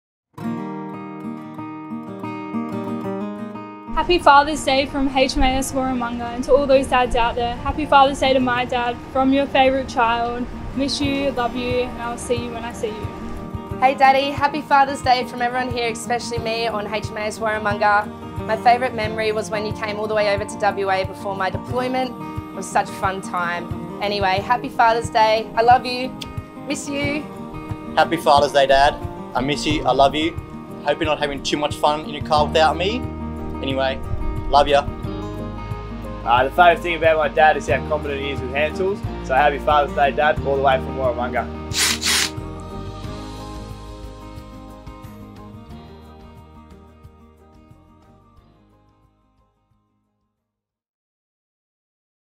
HMAS Warramunga Father's Day Messages
While away on deployment, the ship's company of HMAS Warramunga sent their personal best wishes back home to loved ones for Father's Day 2021.